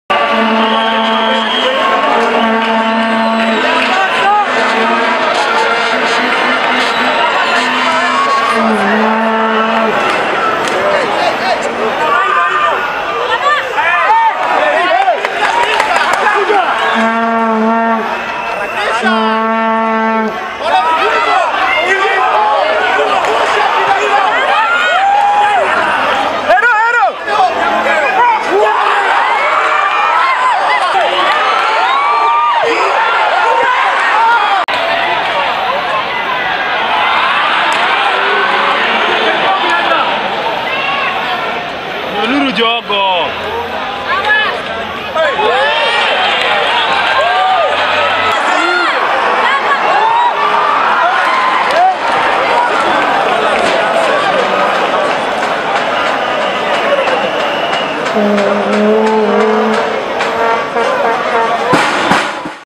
Top 8 Gor vs Tusker Fc
Gor Mahia vs Tusker FC at Nyayo National Stadium on 08 May 2013 and 19:00. Tusker won by a solitary goal